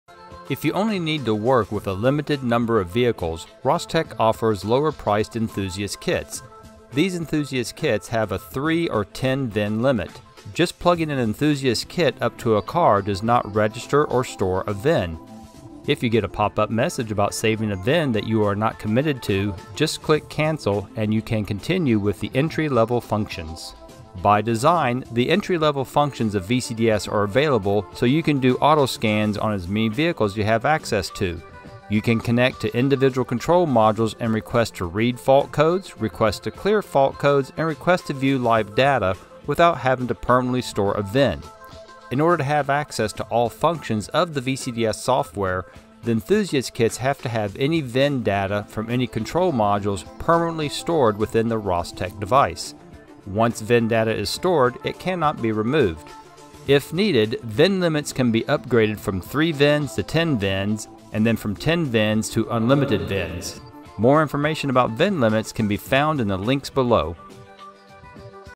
VIN limits
The "Enthusiast" level HEX-V2 and HEX-NET with the 3VIN or 10VIN limit devices can be plugged up to an unlimited number of compatible vehicles and have some entry level functions without registering any VIN data.  More details about VIN limits are found here:

For registered customers who need to upgrade their VIN limits, here is the order page: